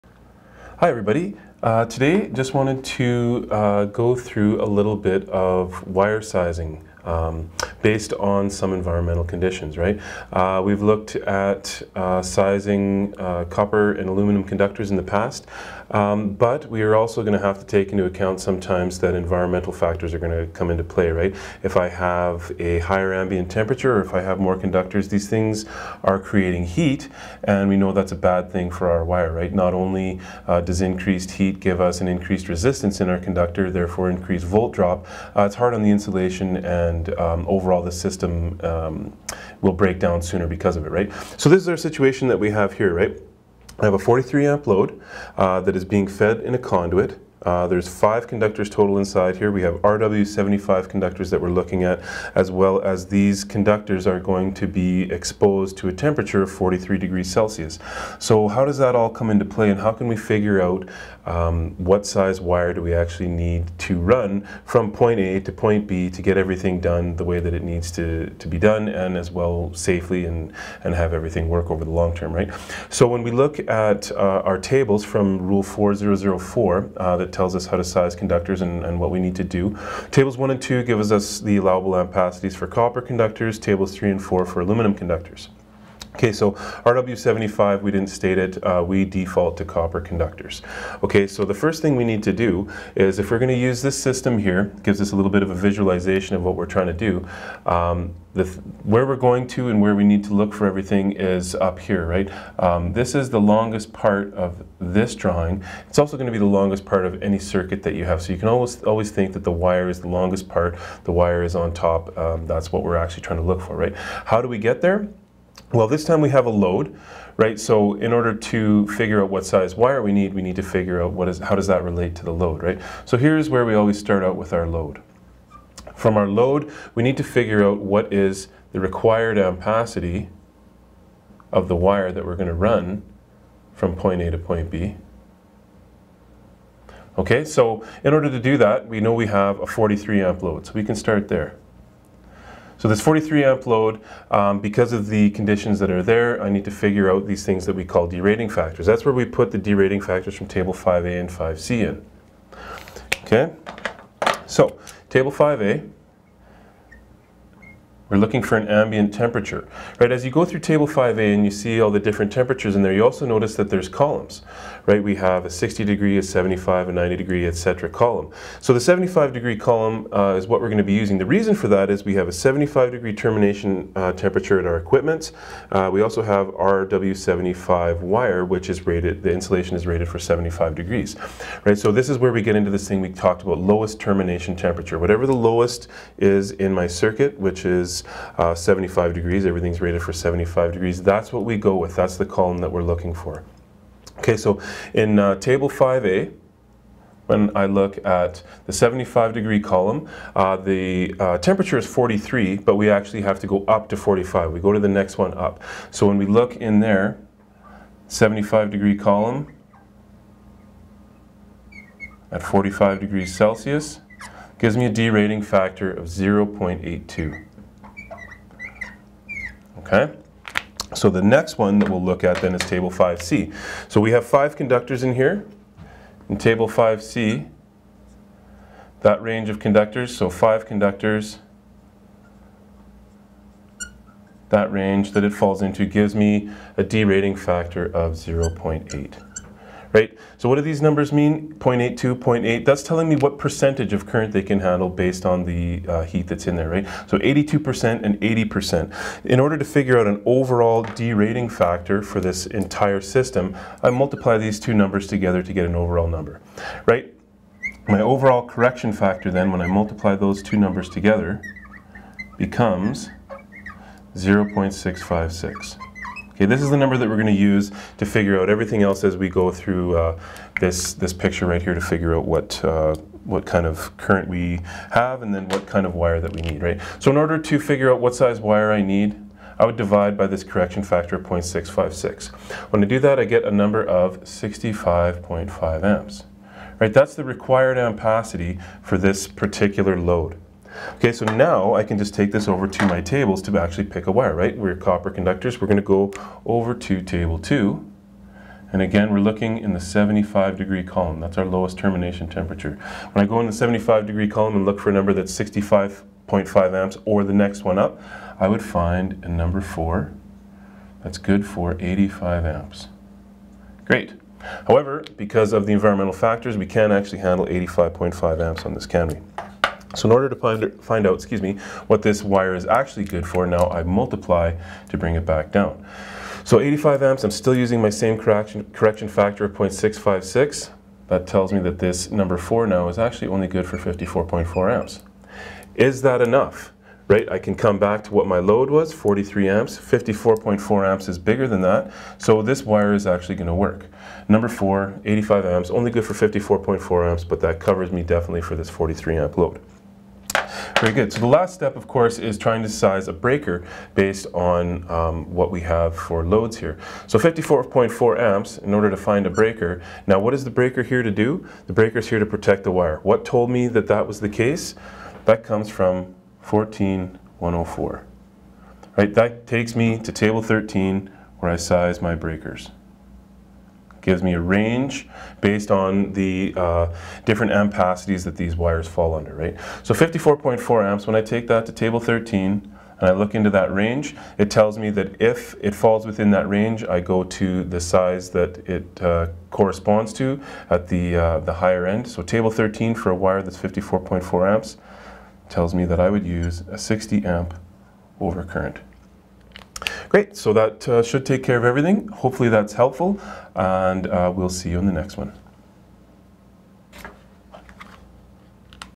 Conductor sizing 1
Thank you to M.Goma for pointing out a calculation error:
@ 5:30 the calculation should come out to 55.76 Amps, not 54.4 Amps.

Sizing conductors based on rule 4-004 of the 2015 CEC, applying de-rating factors from Tables 5A and 5C.